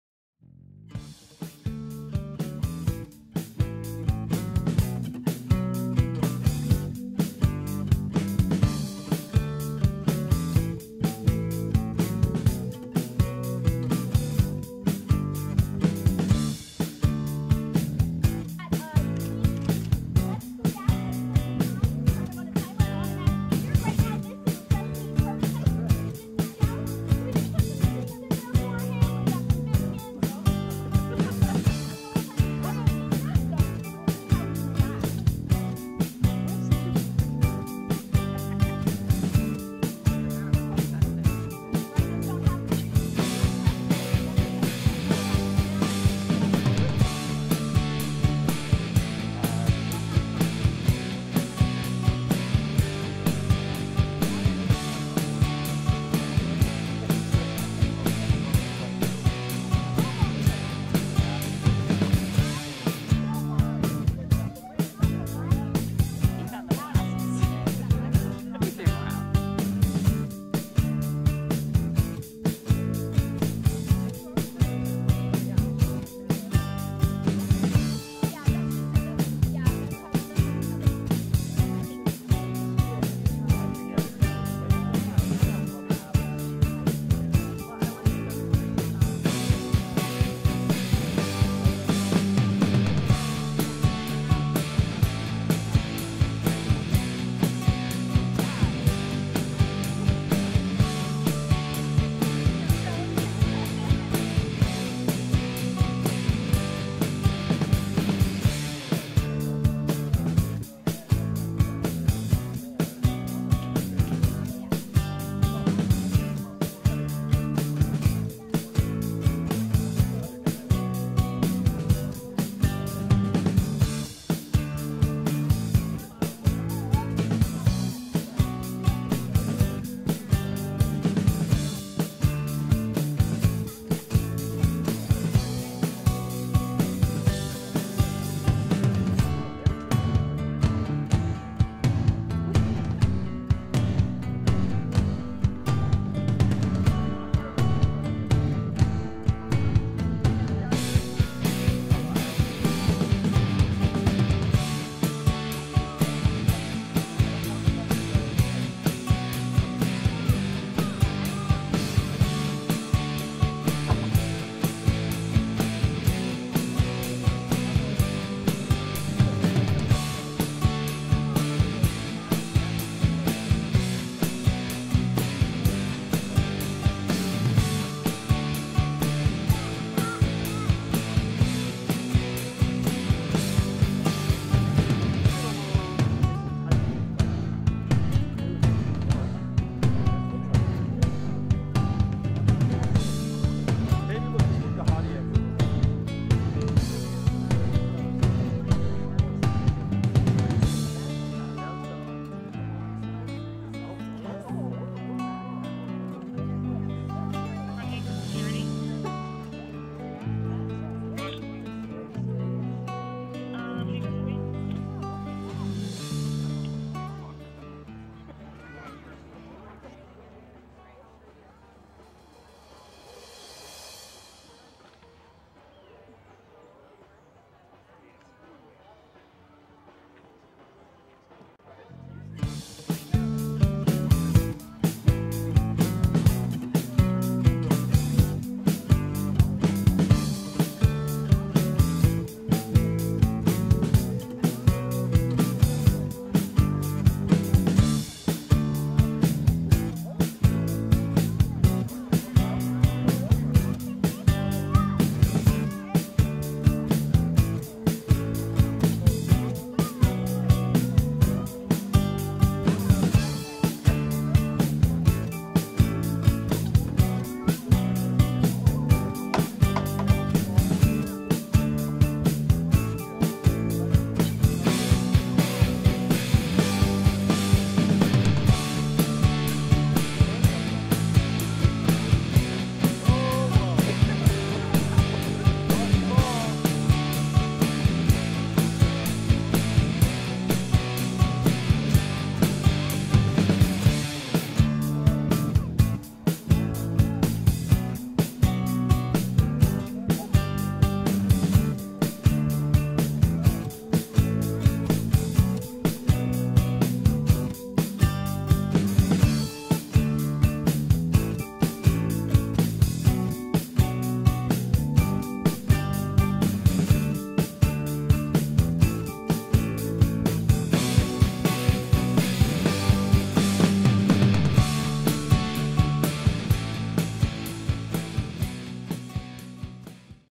Boone meets the rhino Foobler
As part of the World Rhino Day celebration at San Francisco Zoo, we introduced Boone and Gauhati to their new toy- a custom-designed time delay puzzle feeder based on the Foobler dog toy!